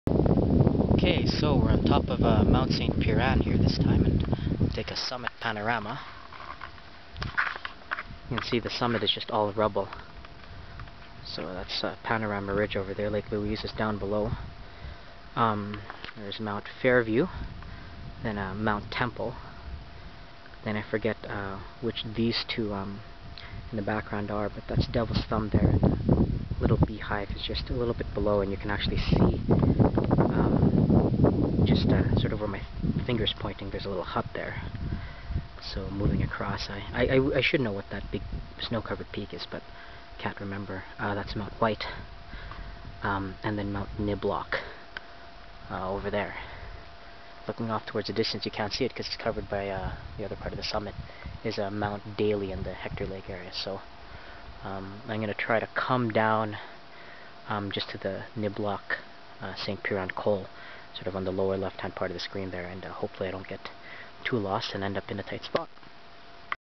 Mt. St. Piran - June 27, 2010
Even though it's not a major peak, this was my first successful solo. I want to come back and do this in the winter time.